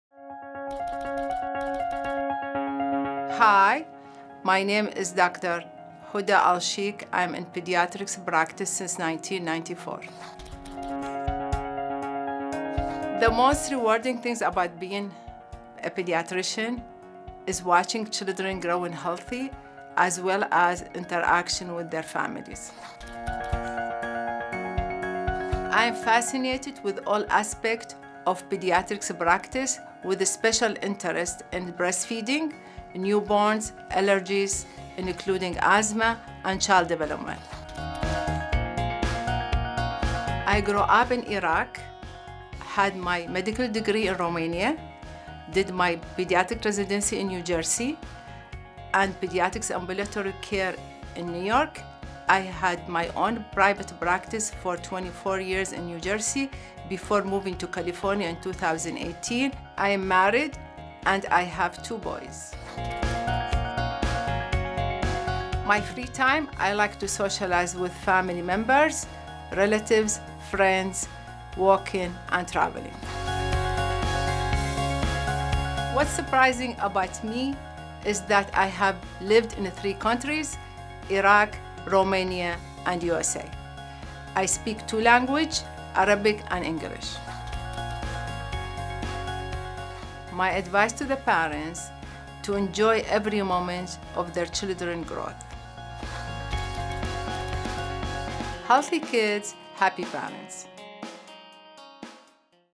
Huda Alsheikh, MD
Meet Dr. Huda Alsheikh, a pediatrician with Children's Physicians Medical Group (CPMG), practicing pediatrics in the East County neighborhood of La Mesa. Find more information about Dr. Alsheikh and all the CPMG pediatricians